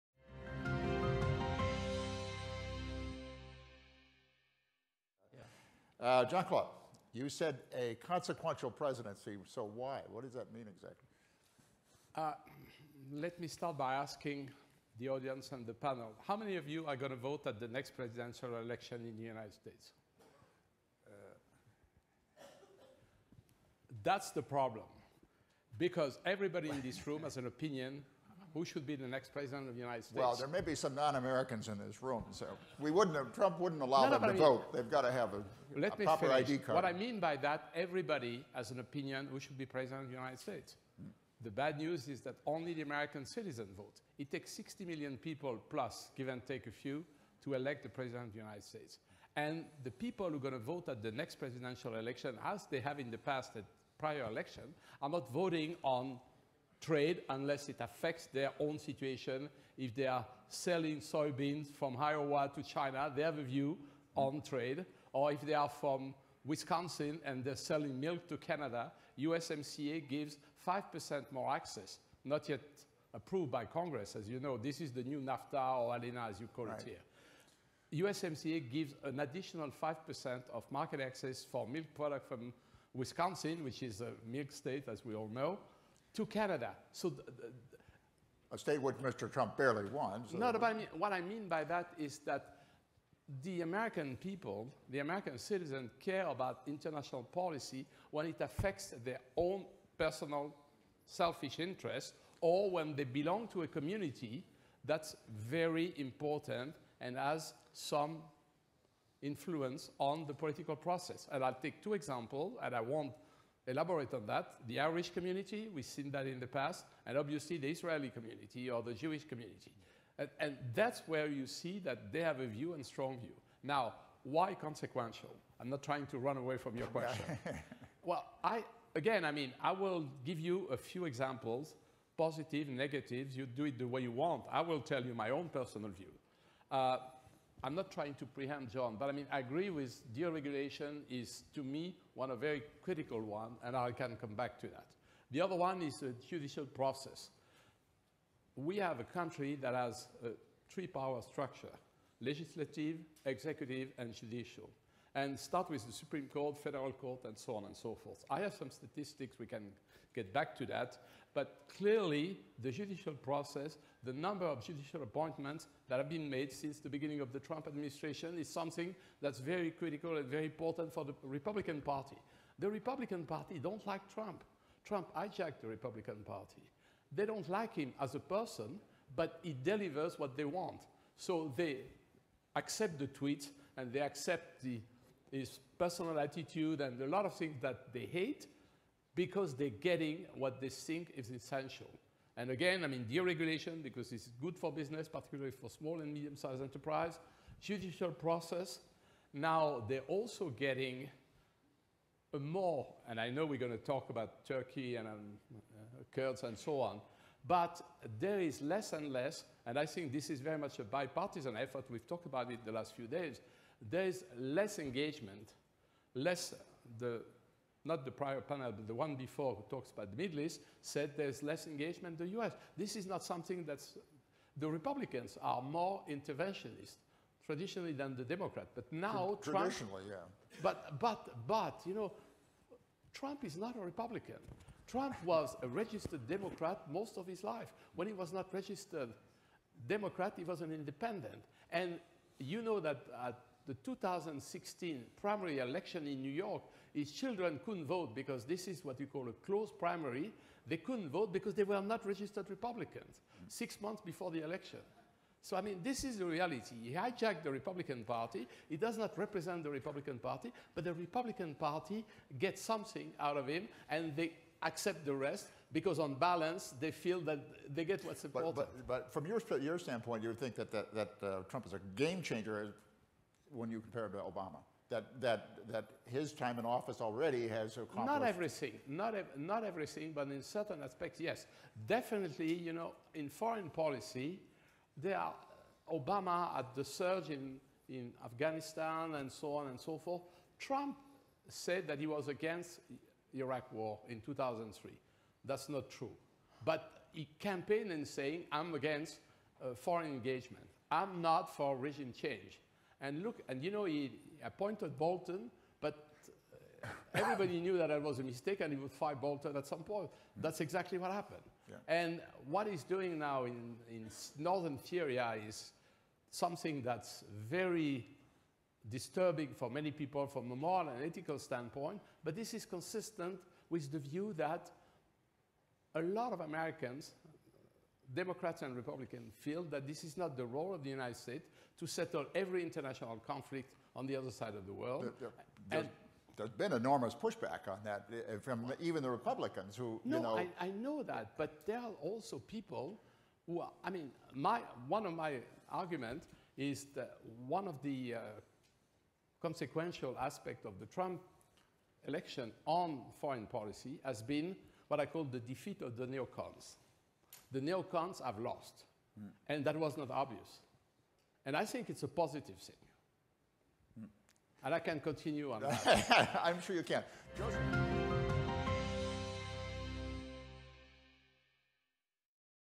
WPC 2019 - Jean-Claude Gruffat - Session 15: The consequences of Trump, Act III
Jean-Claude Gruffat is Chairman of the Competitive Enterprise Institute, Washington DC.